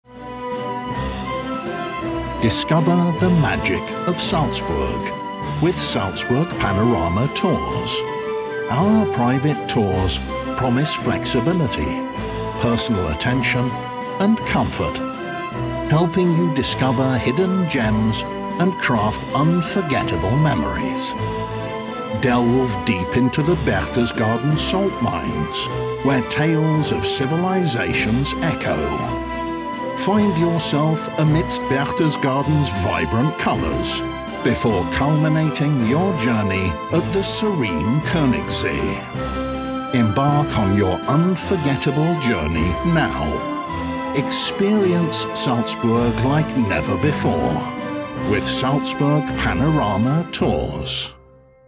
Private Salt Mines Berchtesgaden Tour by Salzburg Panorama Tours | Famous Sight in Berchtesgaden
Welcome to an underground adventure like no other – the Private Salt Mines Tour by Panorama Tours! Join us for an exclusive journey into the fascinating world of the Berchtesgaden Salt Mines, where history, nature, and unique experiences converge.

🕳️ Discover Hidden Treasures
🏔️ Scenic Salt Lake
🚗 Private Transportation
🌄 Breathtaking Bavarian Views
🛶 Boat Rides and Slides

00:00 private pick-up at your hotel
00:14 drive through beautiful Bavarian Mountains
00:19 visit the Berchtesgaden Salt Mines
00:29 explore Berchtesgaden
00:38 visit the Kings Lake (Königssee)

Book your Private Salt Mines Berchtesgaden Tour today for an unforgettable subterranean experience. Get ready to explore a world beneath the surface!